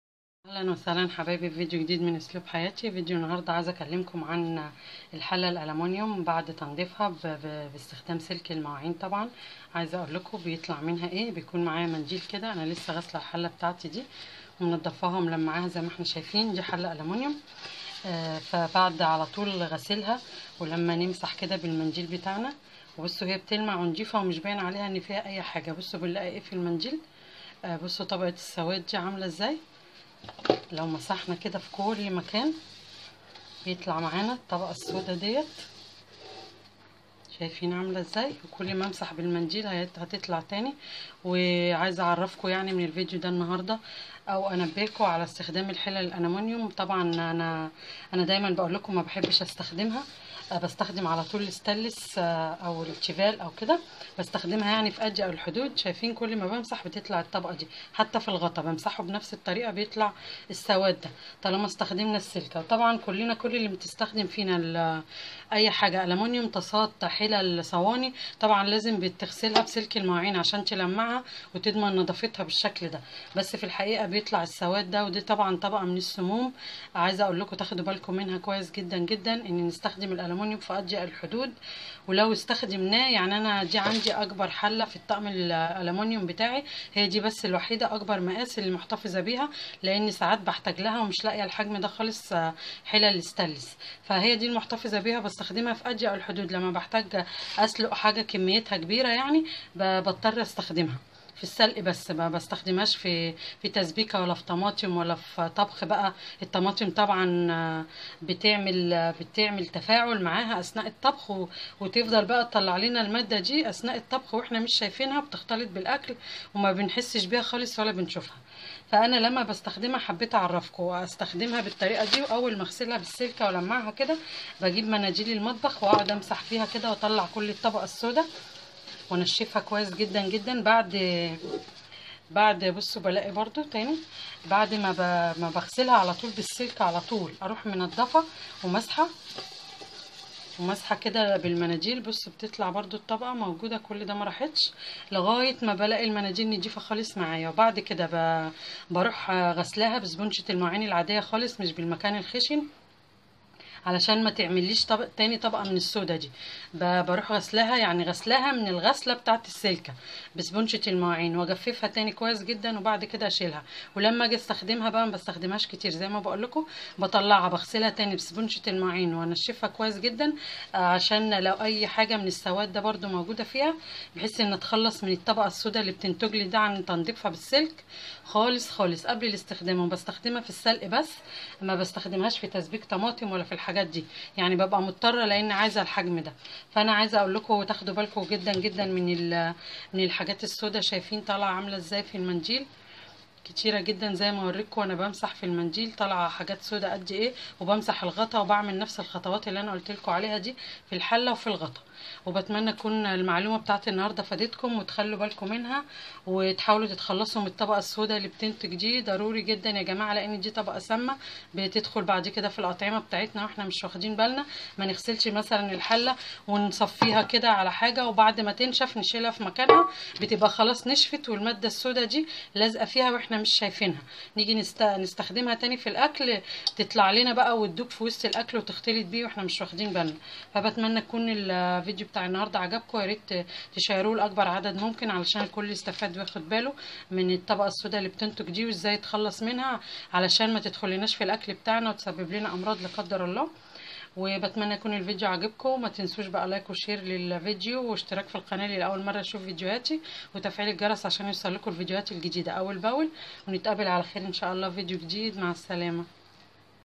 شاهدى ماذا يحدث بعد تنظيف الحلة الألمونيوم بسلك المواعين/خطيييير جدااا 😱😱اسلوب_حياتى_مع_ميرفت
شاهدى ماذا يحدث بعد تنظيف الحلة الألمونيوم بسلك المواعين 😱😱
شيء خطيييير يحدث بعد تنظيف الحلة الألمونيوم بسلك المواعين //شاهدى ما هو !!!؟؟؟

رابط صفحتى على الفيس بوك :
بتمنى تتابعوا الصفحة 😍 😍 😙 ❤ 💙 💚